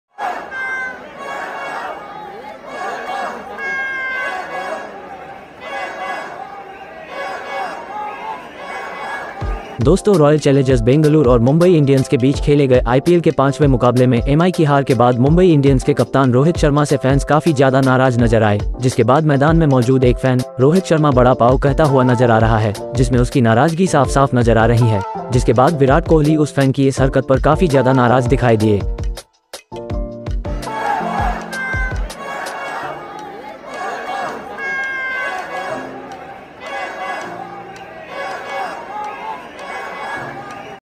Rohit Sharma angry when fan Teasing him by chanting Vada Pav | RCB vs MI IPL 2023
Friends, after the defeat of MI in the fifth match of IPL played between Royal Challengers Bangalore and Mumbai Indians, the fans were very angry with MI captain Rohit Sharma, after which a fan present in the field Rohit Sharma was seen saying Bada Pav In which his displeasure is clearly visible